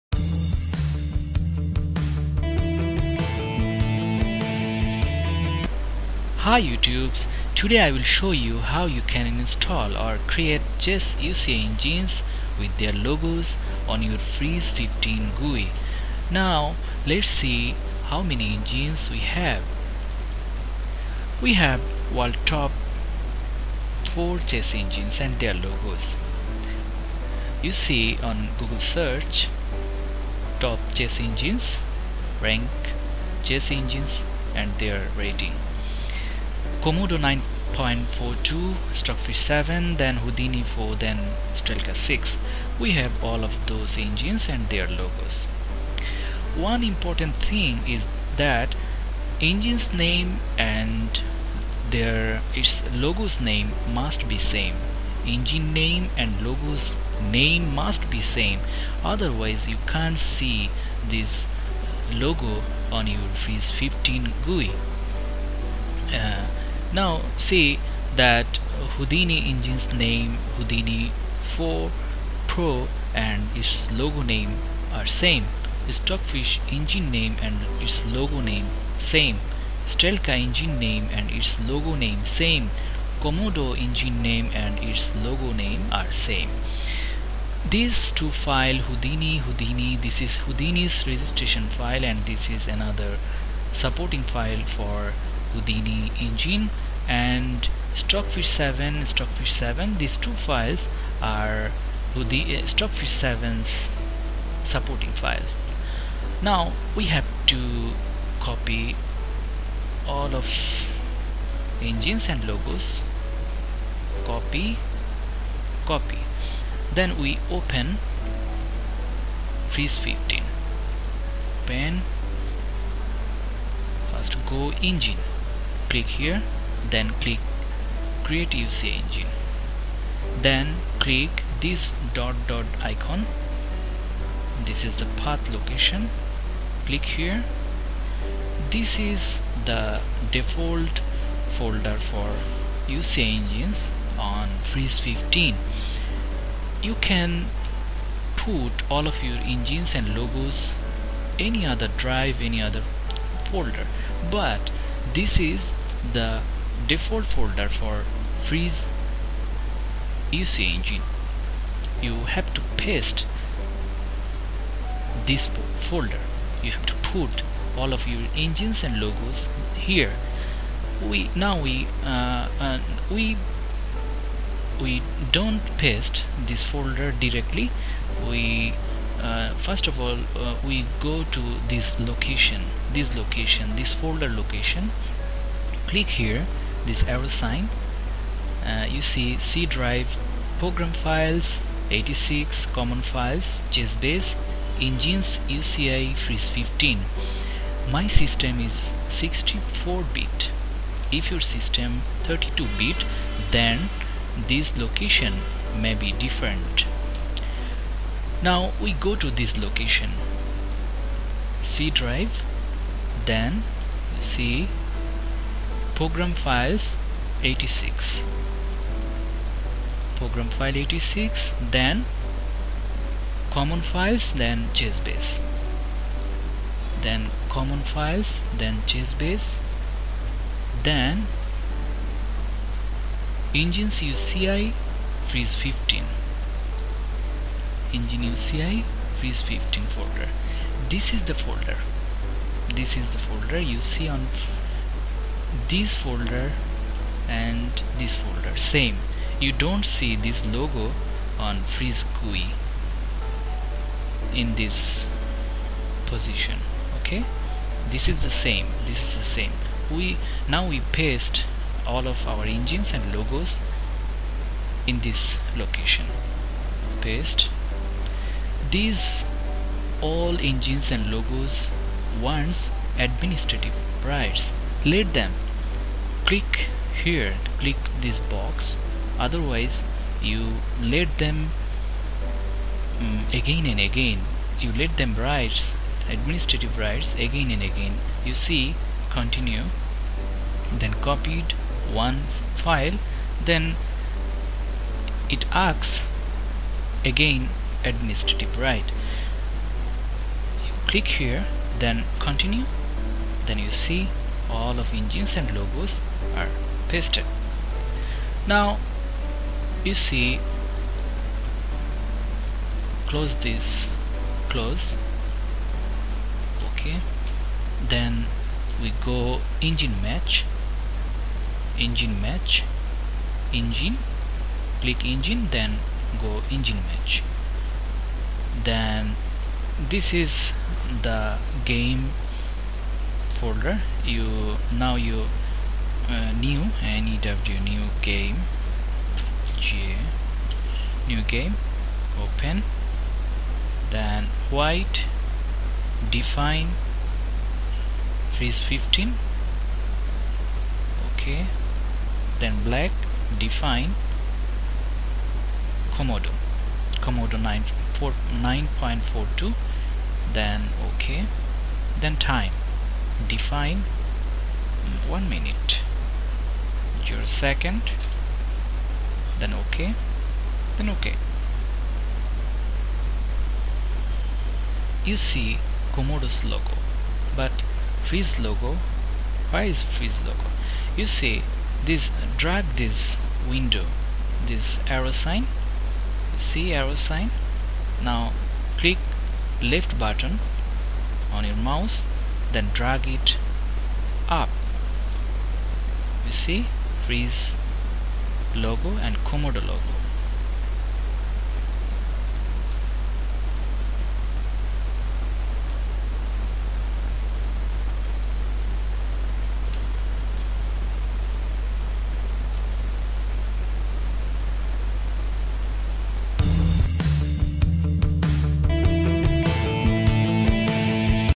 Chess Uci Engines & Their Logos : How to install on Fritz 15
A Tutorial About Chess Gui (Graphical user interface) Fritz 15